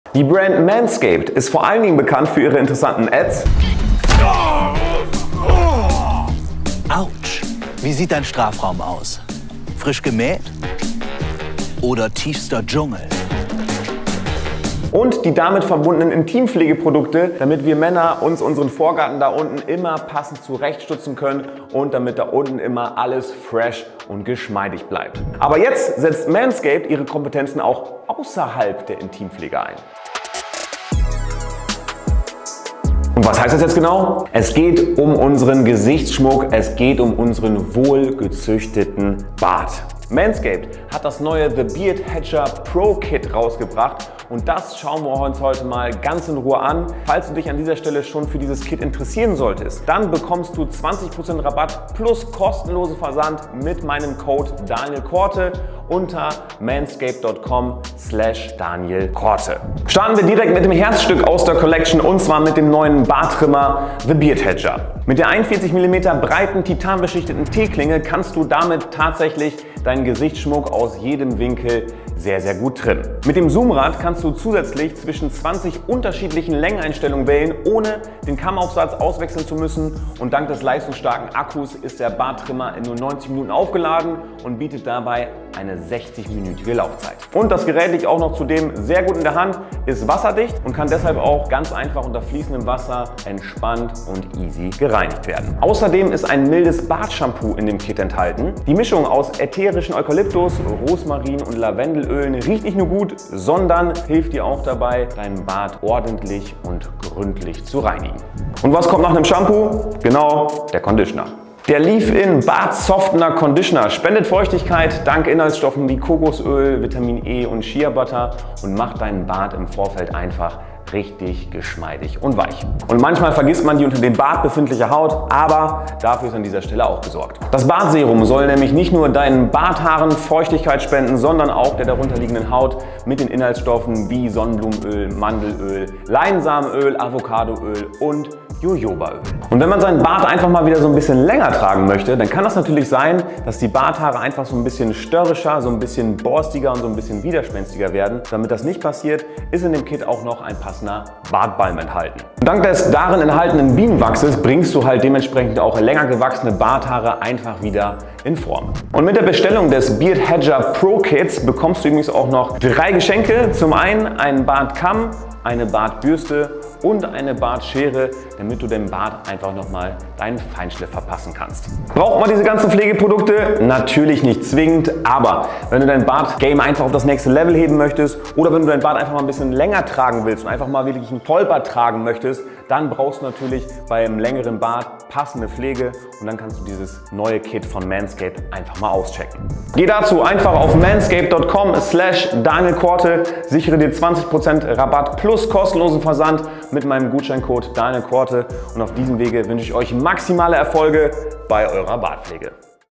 Vom Vorgarten in dein Gesicht: Der neue Barttrimmer von Manscaped!
Hey Leute,
auf meinem Kanal gab es schon den einen oder anderen Beitrag zum Thema "Männerpflege", in Kombination mit der Marke "Manscaped". Manscaped ist vorallem für ihre hochwertigen Intimpflege-Produkte bekannt. Egal ob es ein Trimmer für die Intimpflege, ein Shampoo oder Boxershorts sind, bei Manscaped werdet ihr fündig. Doch mittlerweile stellt Manscaped nicht nur Pflegeprodukte für die Intim- und Körperpflege zur Verfügung. Denn Manscaped bringt mit dem "The Beard HedgerTM Pro Kit", einen neuen Barttrimmer auf den Markt. In dem "The Beard HedgerTM Pro Kit", ist nämlich der neue "The Beard Hedger" enthalten und den schauen wir uns in diesem Video gemeinsam an. Natürlich gehe ich auch auf die weiteren Produkte ein, welche im "The Beard Hedger Pro Kit" enthalten sind, wie zum Beispiel das Bartöl, das Bartshampoo und den Bartconditioner von Manscaped. Das "The Beard Hedger Pro Kit" von Manscaped ist wirklich eine Ansage und für Jeden interessant, welcher seine Bartpflege auf das nächste Level heben möchte. Für Leute die viel Wert auf die Bartpflege legen, ist das "The Beard HedgerTM Pro Kit" sicherlich einen Blick wert. Schreibt gerne in die Kommentare, was ihr vom "The Beard HedgerTM Pro Kit" von Manscaped haltet und ob das "The Beard HedgerTM Pro Kit" auch etwas für euch wäre. Ich wünsche euch weiterhin viel Spaß, beim Schauen meines Videos zum "The Beard Hedger Pro Kit Review" bzw. "The Beard HedgerTM Pro Kit Test".